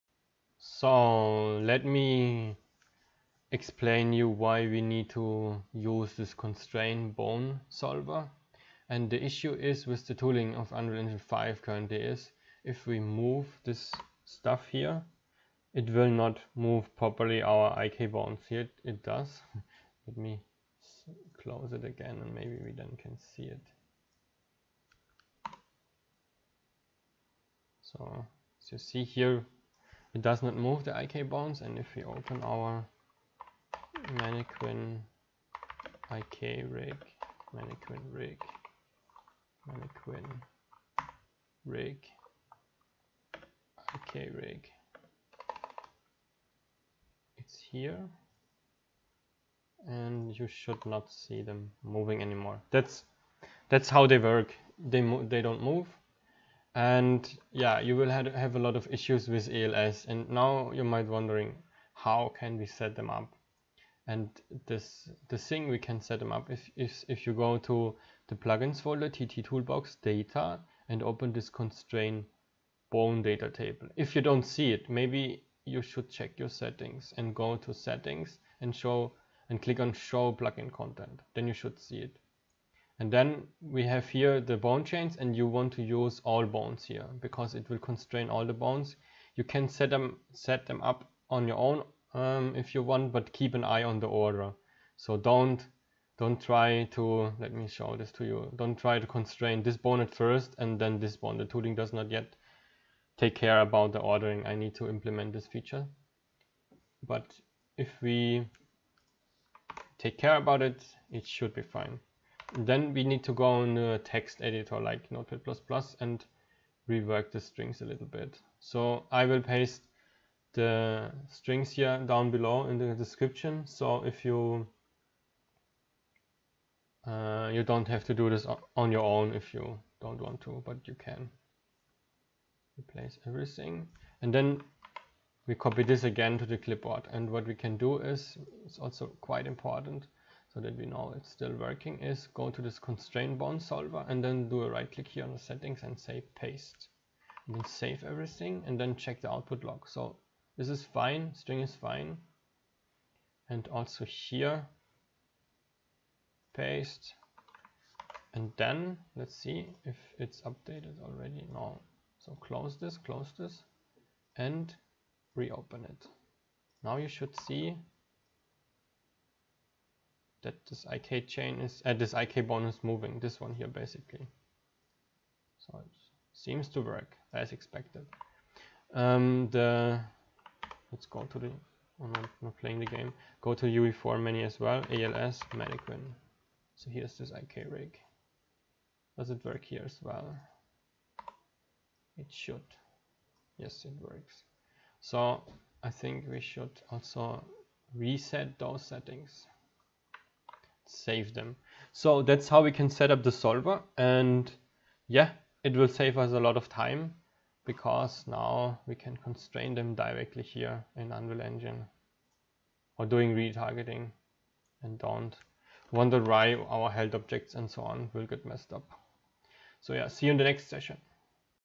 #5 Constraint IK Bones - Retarget Paragon TwinBlast to ALS - UE5
Within this session we will setup the solver to constraint the ik bones.

This string was used in the video,

For the entire series was used.